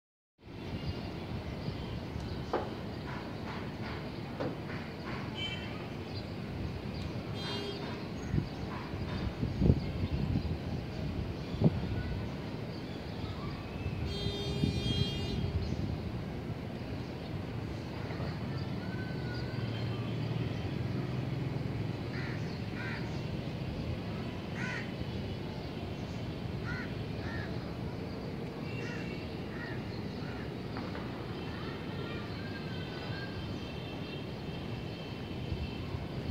Cloud Filtering Sun Rays
Watch the Beautiful Sun Rays when filtered through clouds & the birds flying.